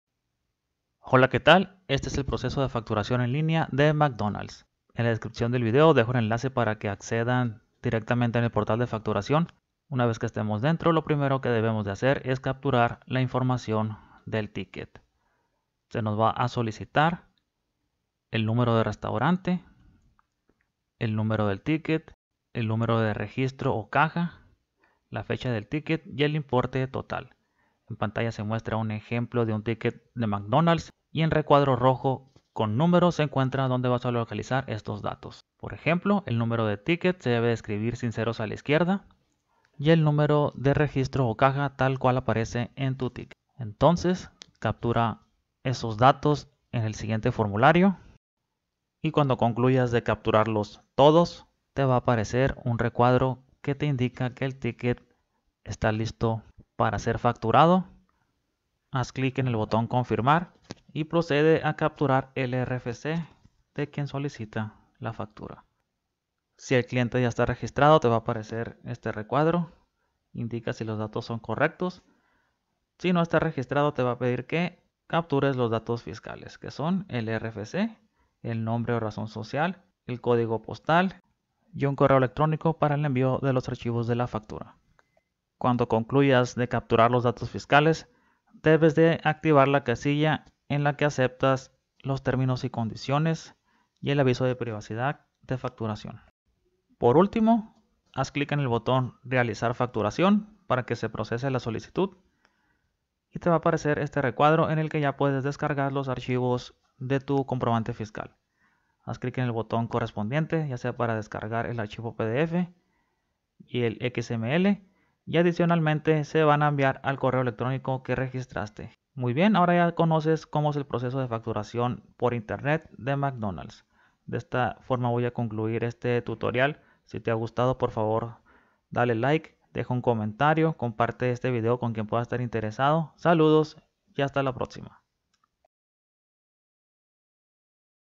¿Cómo Realizar La Facturación De Tickets De McDonalds? - Facturación Online Restaurantes McDonald's
Este es el proceso de facturación en línea de Restaurantes McDonald's, ingresa en la siguiente página web

después de ingresar, ya puede comenzar a facturar los tickets de consumo hechos en restaurantes McDonalds.